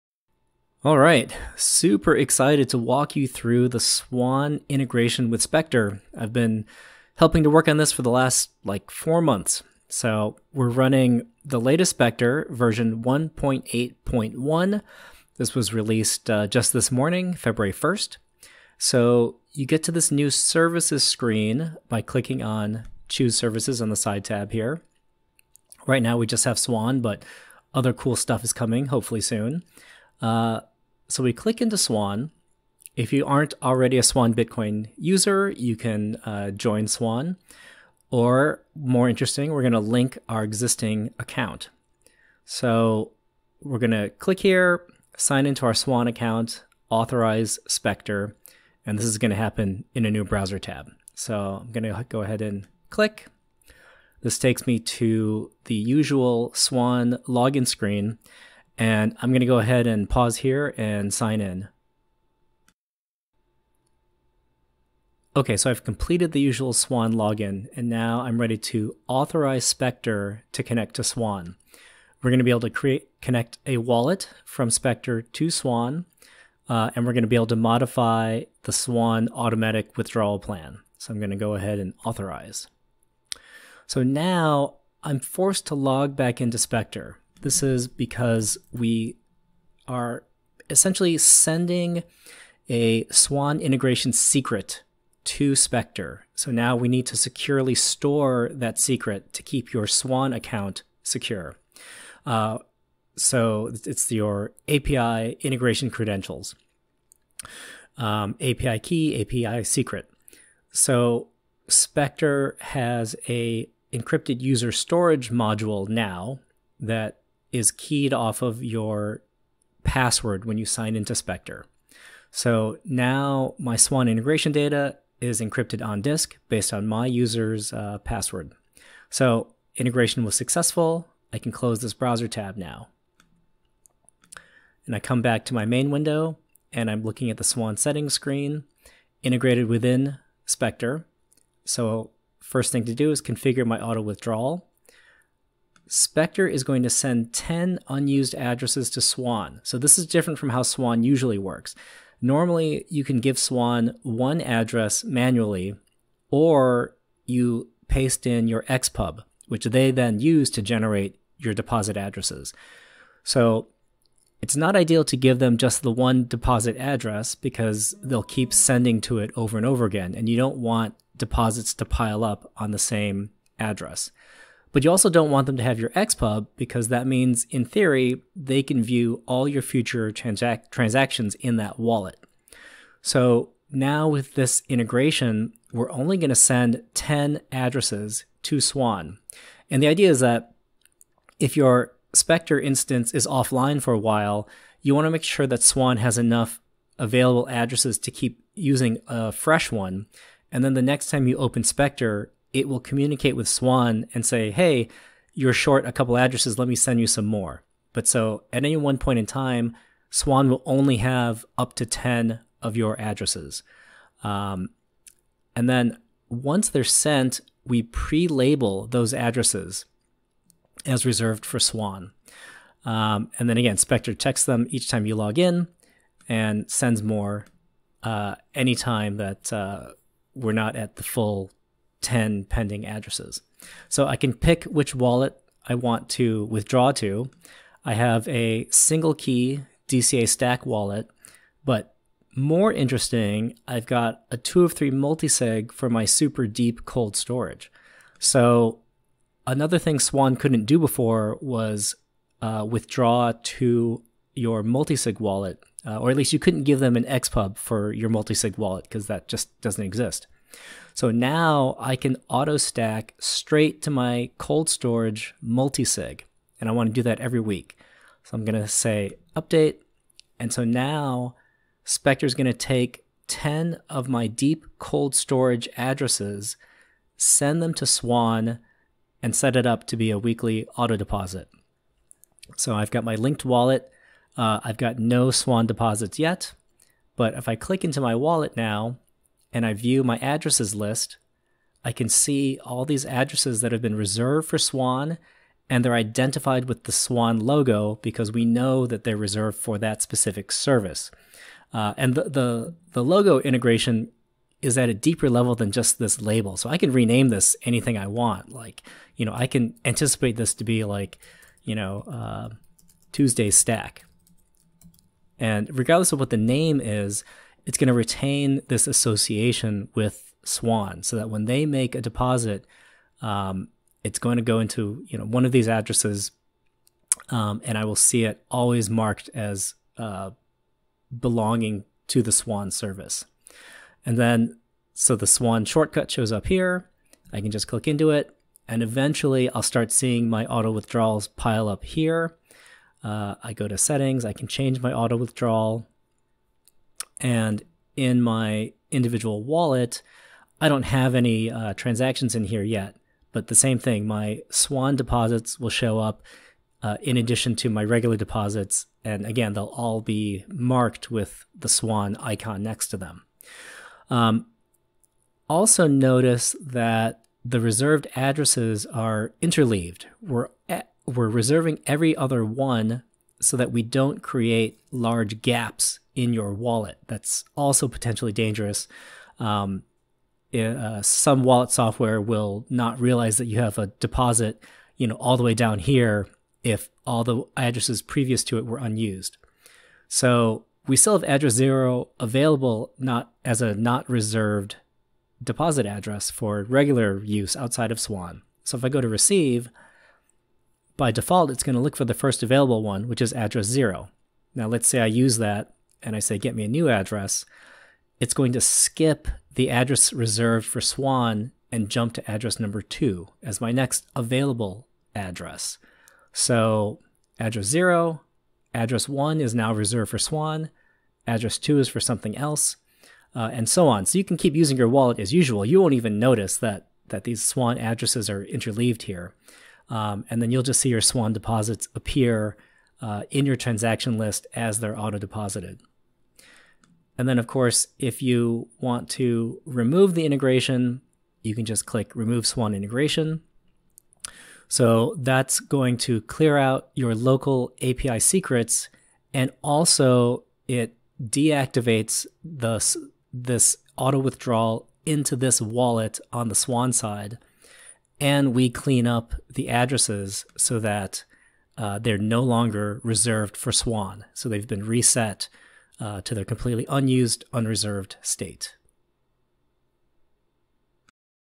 Swan Bitcoin integration on Specter Desktop
- Auto-stack your DCA straight to your multisig cold storage!
- Complete link to your SwanBitcoin account.
- Swan is receiving only 10 unused addresses. tldr: NO XPUB!!
- Addresses sent to SwanBitcoin are "reserved" in your Specter Wallet in anticipation of upcoming auto-withdrawals from Swan.
- Also see how you can remove the Swan integration again.

(Keith keeps flipping between saying "auto-withdrawal" and "auto-deposit" — same diff: withdrawing from Swan, depositing to Specter).